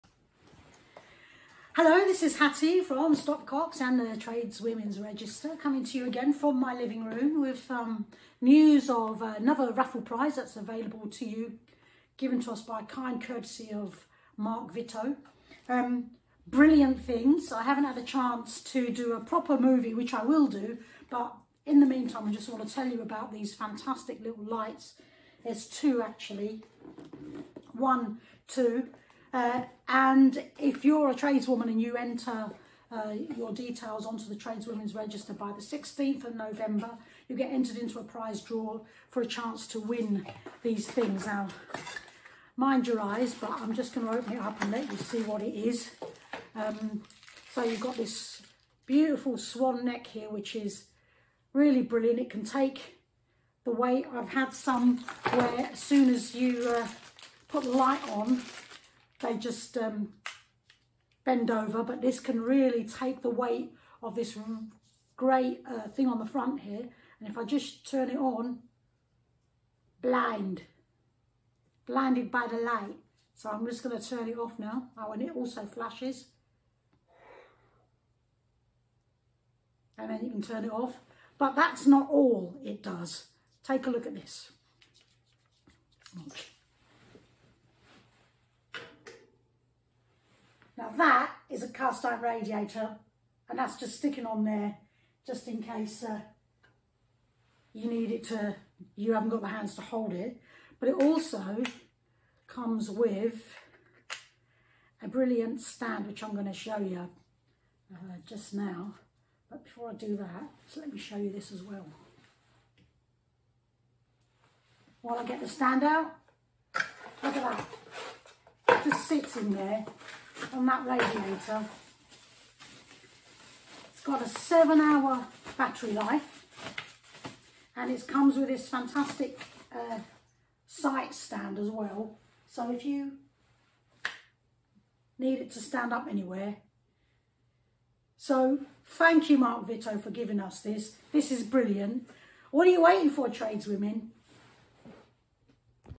Win a set of these brilliant lights from Mark Vitow.
Hattie Hasan MBE tell you how you can be in with a chance of winning these amazing lights. Sign up to the Register of Tradeswomen by 16th November to be entered into the prize draw for brilliant gifts. These two lights from Mark Vitow are amazing.